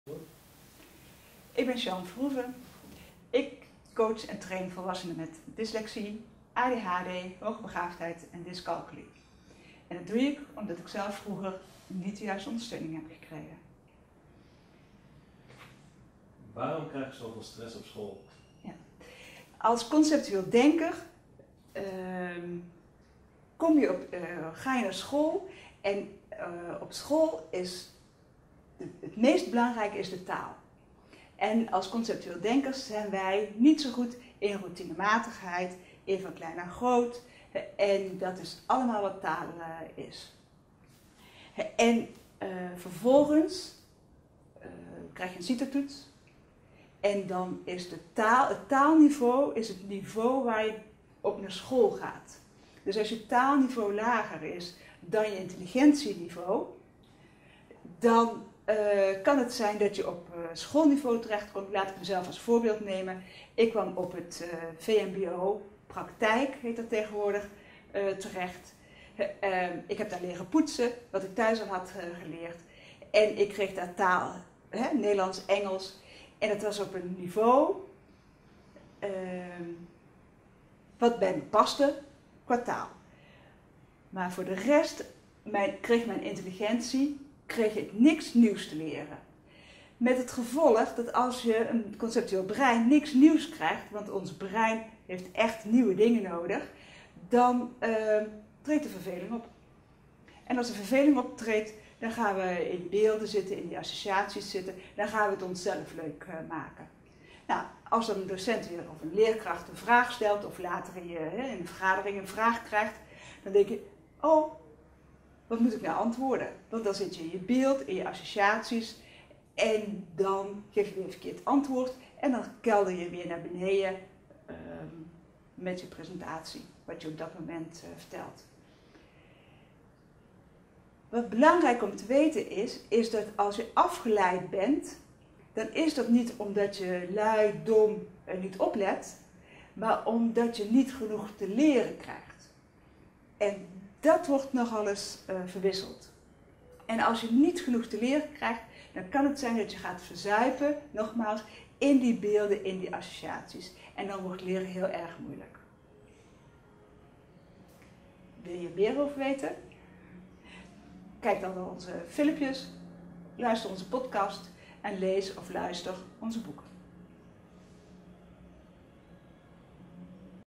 Waarom zoveel stress op school?
Regelmatig zitten conceptueel denkende leerlingen op een lager onderwijsniveau dan ze qua intellect aankunnen. Dat komt onder andere door de nadruk die er op taal wordt gelegd. Het gevolg is dat zij zich vaak vervelen op school en ook dat geeft stress! Meer weten? Kijk op